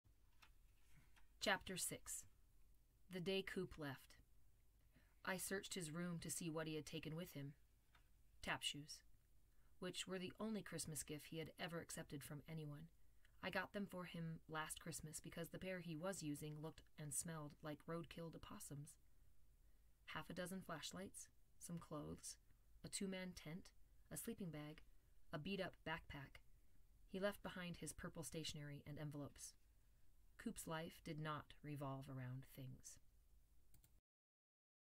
Ch6 Beneath by Roland Smith
Ch6 Beneath by Roland Smith (Read aloud by Mrs. Hammitt.)